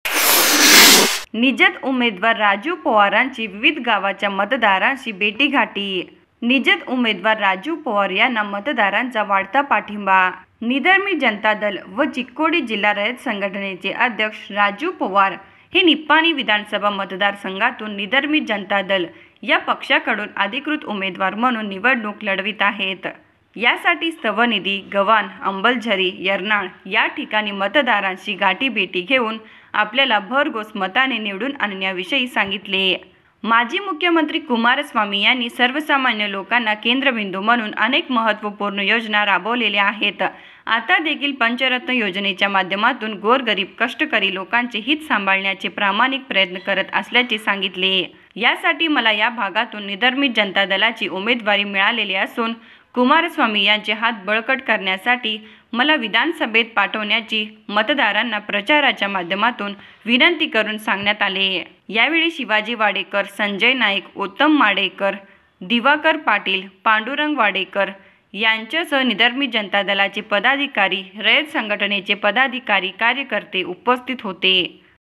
निपाणी : राजू पोवार यांच्या विविध गावांना गाठीभेटी
निपाणी : राजू पोवार यांच्या विविध गावांना गाठीभेटी (ICB NEWS NIPANI)